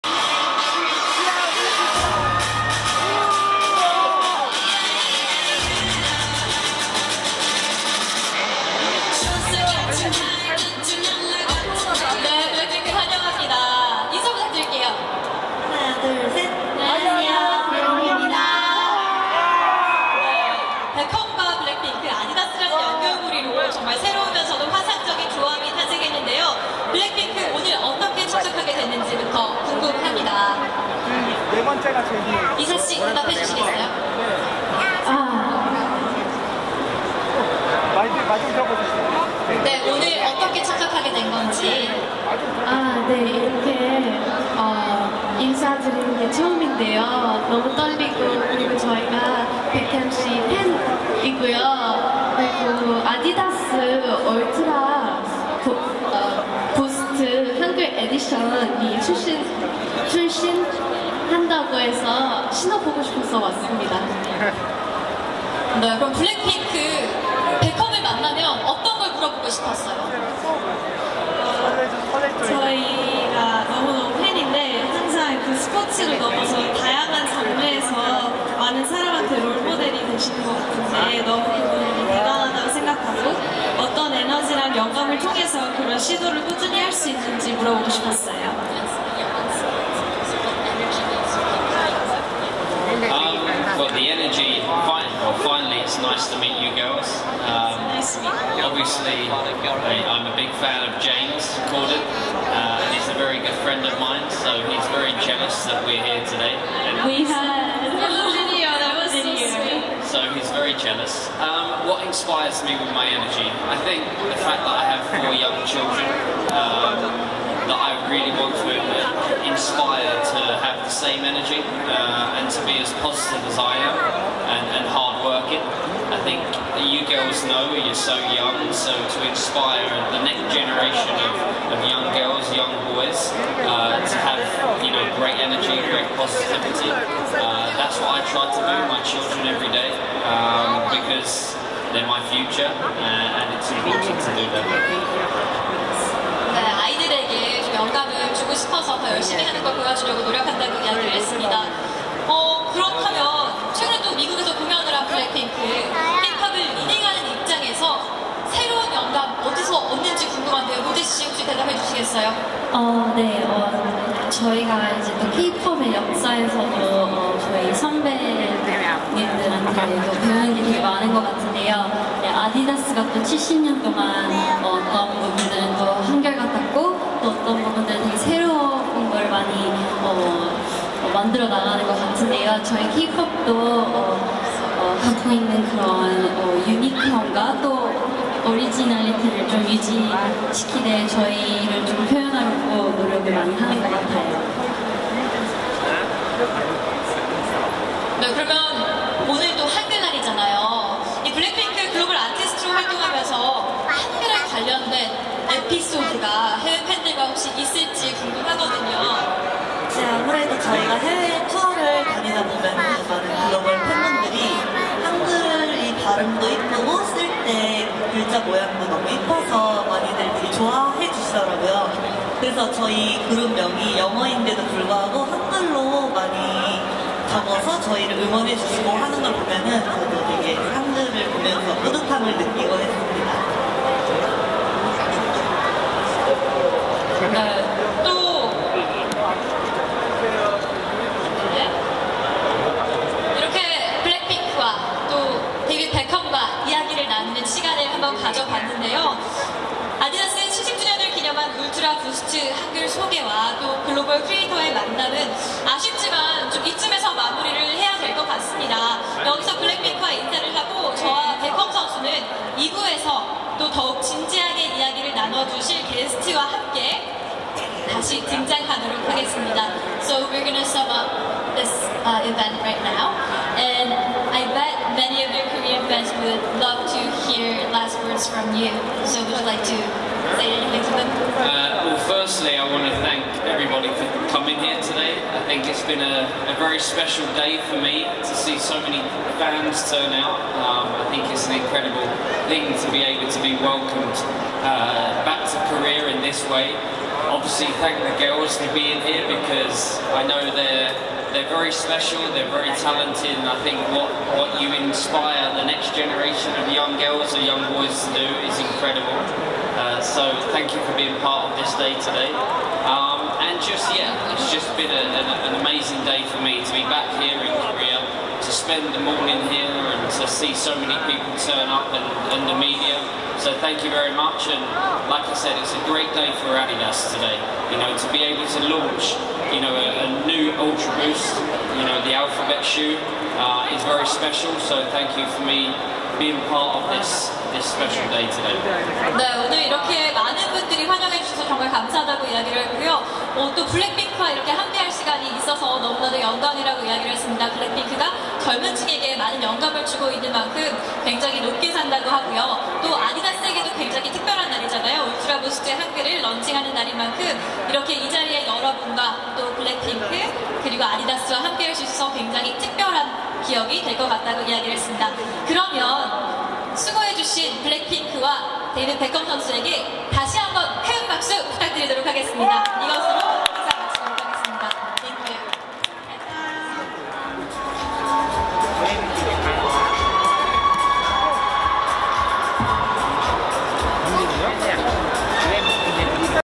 베컴과 블랙핑크와의 만남 @아디다스 70주년행사 (19.10.09)
191009 블랙핑크 리사 지수 로제 제니 - 베컴과 블랙핑크와의 만남 (아디다스70주년행사 타임스퀘어) 직캠 fancam by zam